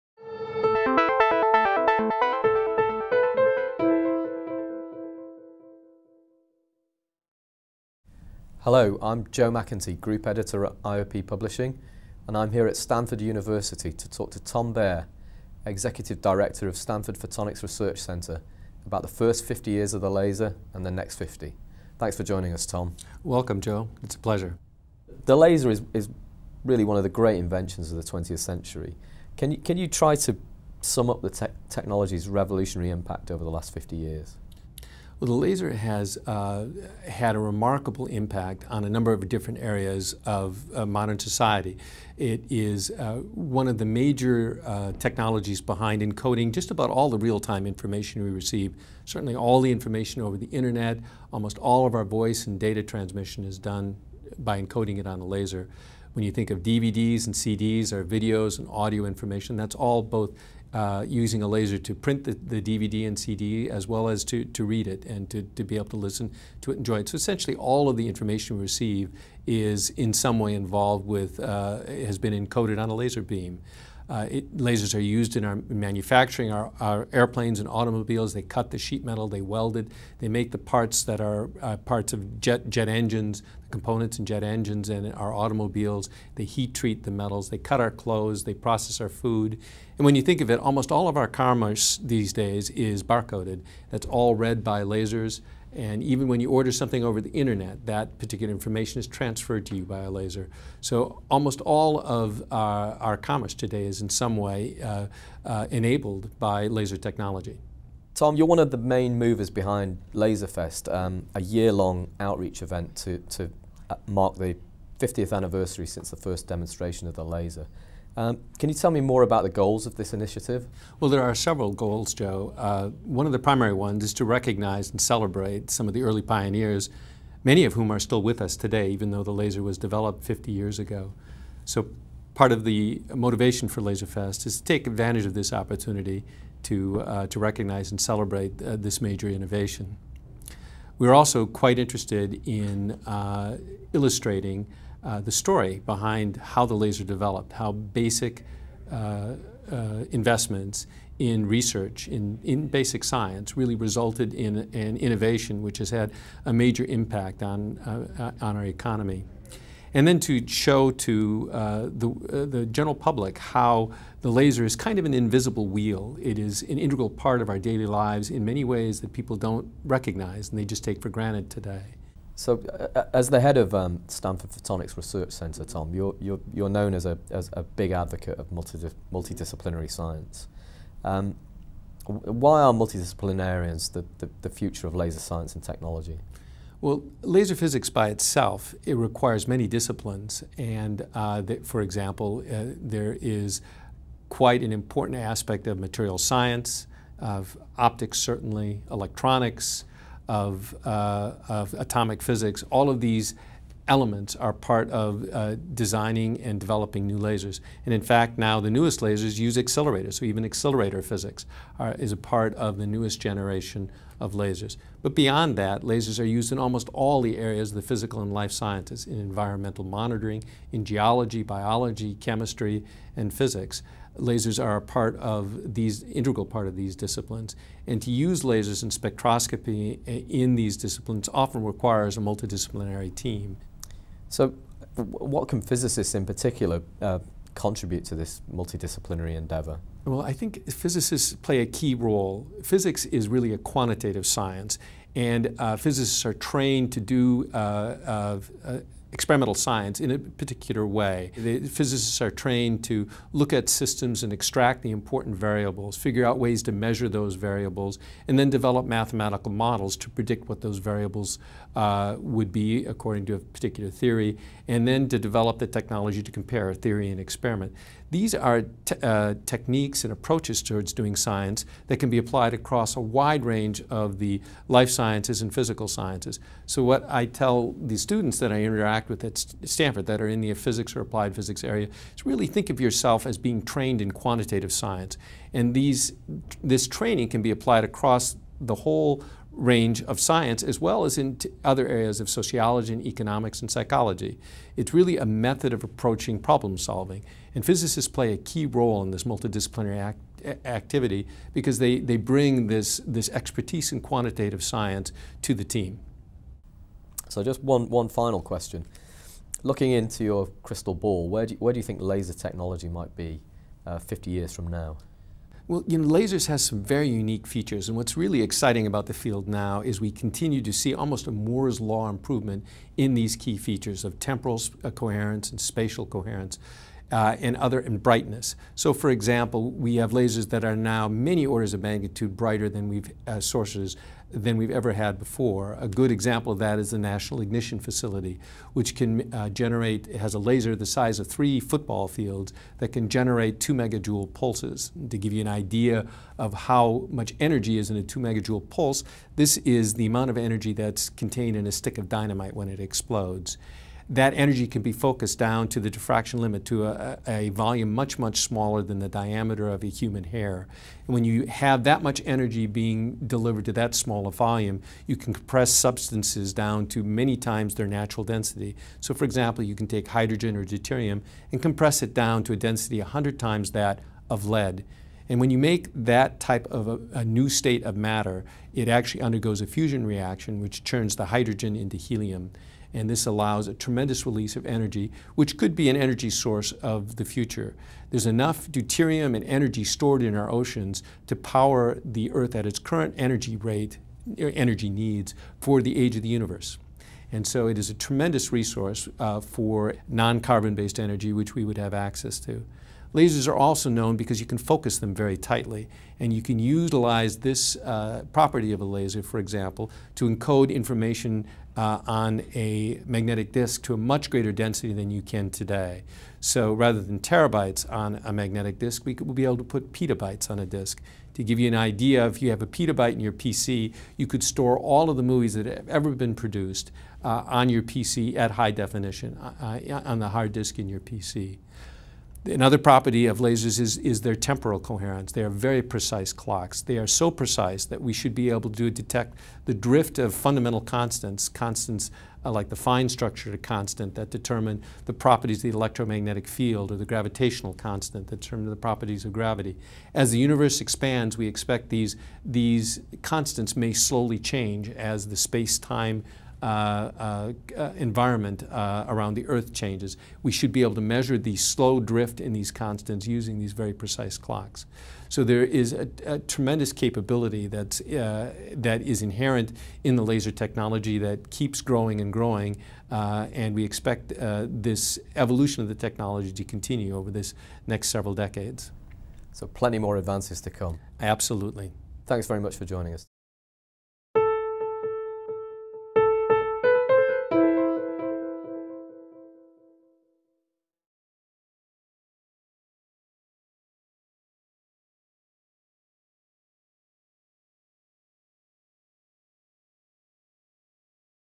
The first 50 years of the laser, and the next 50
Tom Baer of the Stanford Photonics Research Center reviews 50 years of laser physics, and predicts how lasers will develop over the next 50 years. Visit physicsworld.com for more videos, webinars and podcasts.